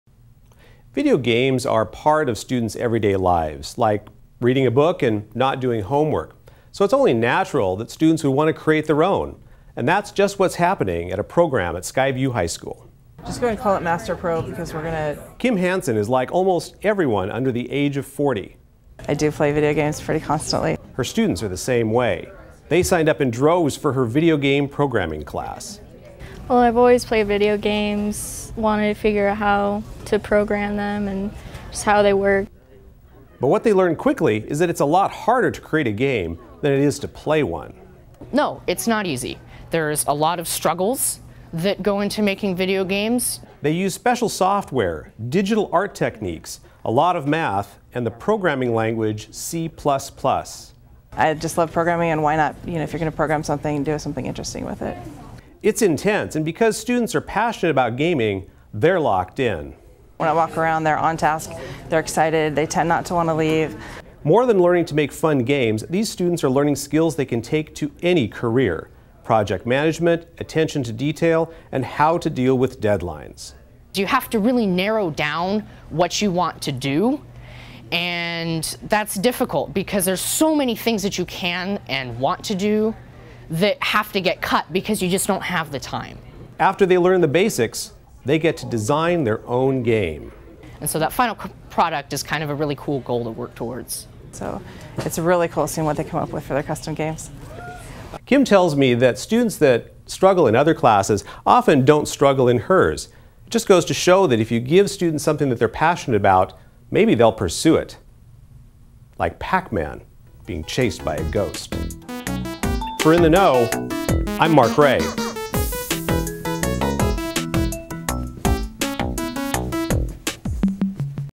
Video Game Class
At Skyview High School, students learn to make their own video games.  Reporter: Mark Ray